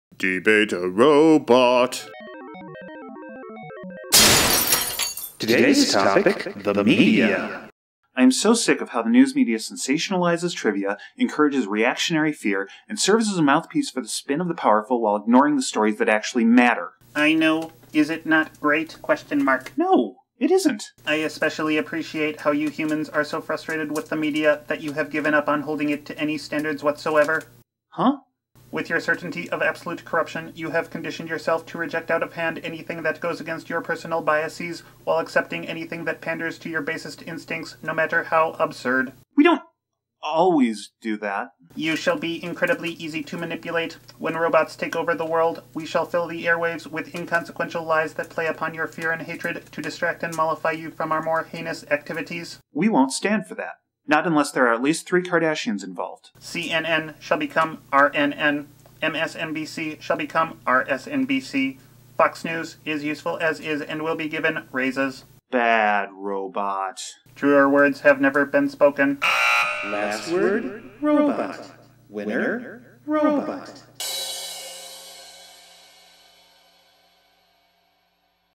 Debate a Robot, Episode 14: The Media
Ah, journalism. One of the most reviled professions, for reasons both just and ludicrous. Today, Rombo and Hiram discuss the media, its sins against actual information, and its usefulness for industrious robots with an eye toward world domination.